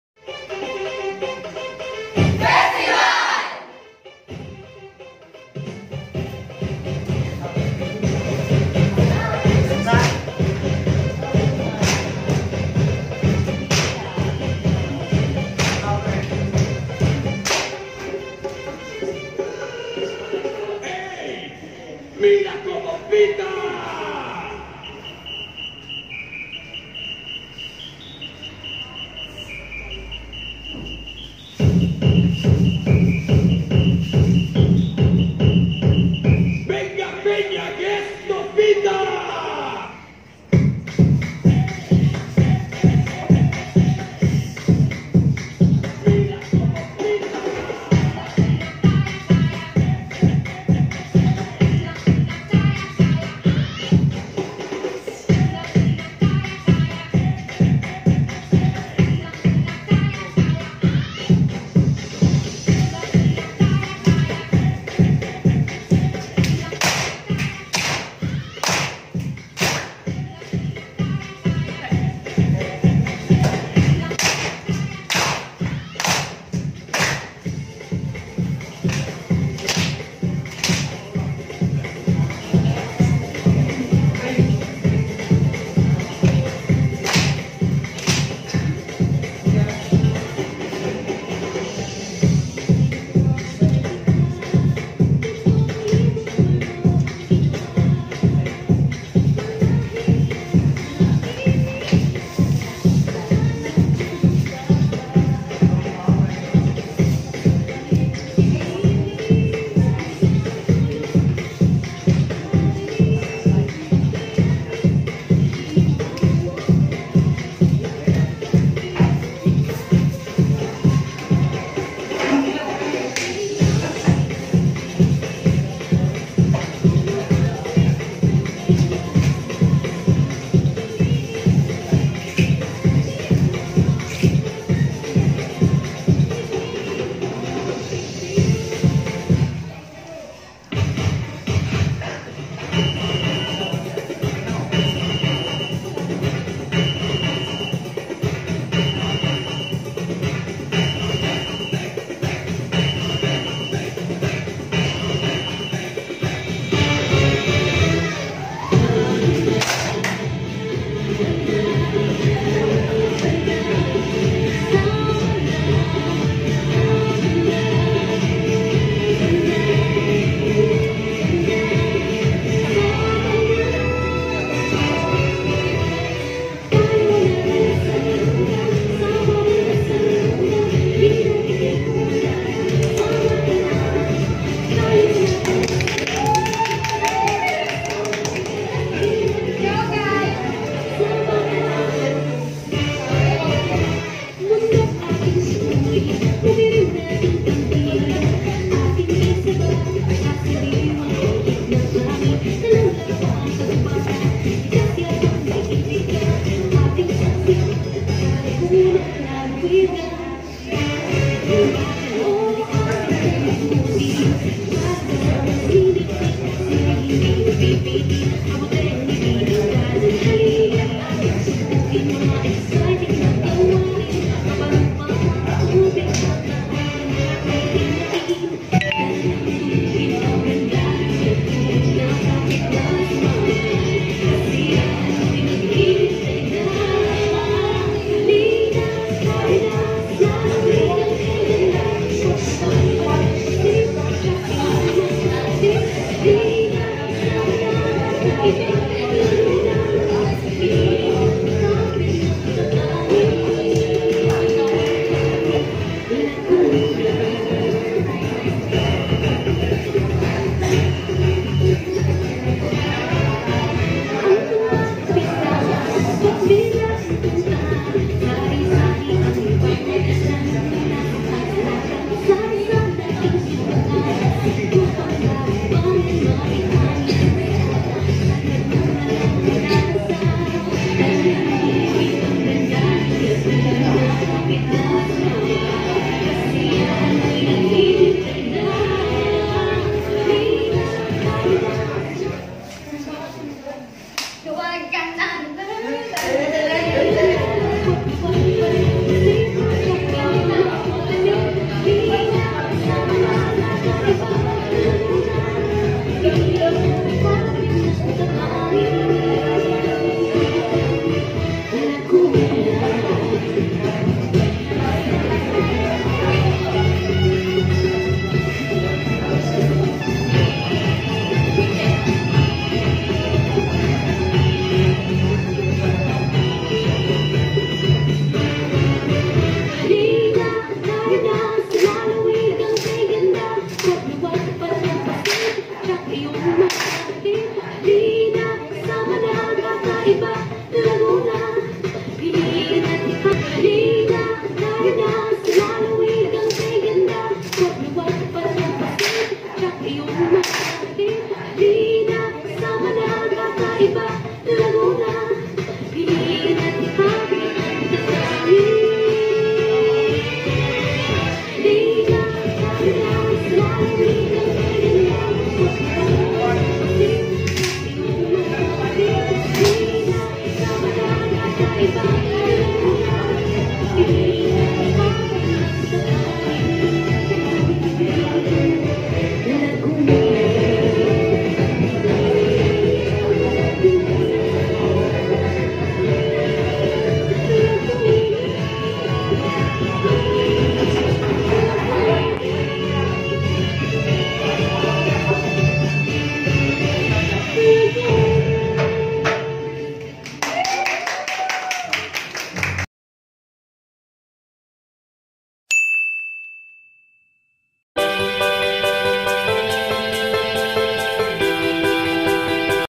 The music stop and look what just happen...
Yow guys! This is the video of our dance performance for the Buwan ng Wika. Kung napanood mo ang last video ko, nagpakita ako ng unting clips ng performance namin. And here it is, the full dance performance just for you guys 😊 AWARD WINNING PERFORMANCE!!! Hope you enjoy!!!

Add/Follow me on this account ;

PEACE! ✨